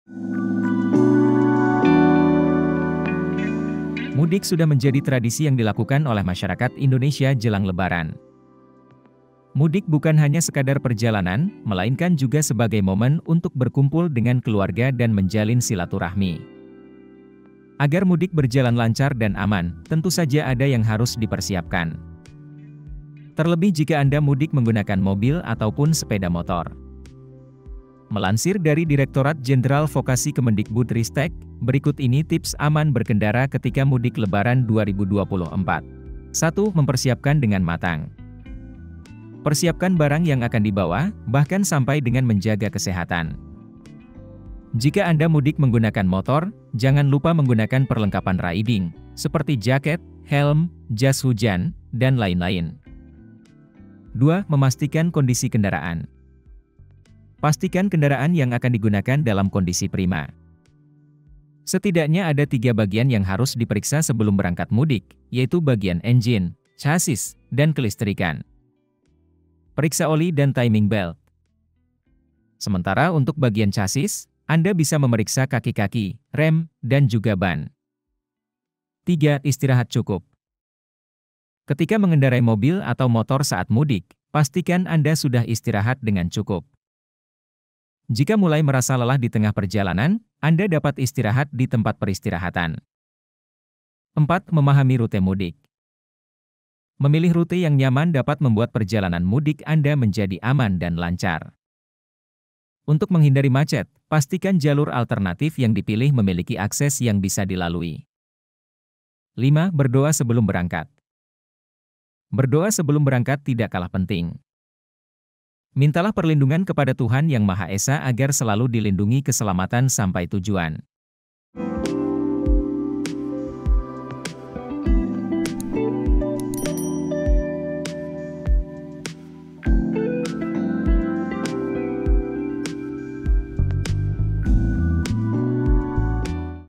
Mudik Lebaran 2024, Ini Tips Aman Berkendara saat Mudik Hari Raya Idul Fitri | Kontan News
Mudik sudah menjadi tradisi yang dilakukan oleh masyarakat Indonesia jelang Lebaran.

Mudik bukan hanya sekadar perjalanan, melainkan juga sebagai momen untuk berkumpul dengan keluarga dan menjalin silaturahmi.

Agar mudik berjalan lancar dan aman, tentu saja ada yang harus dipersiapkan.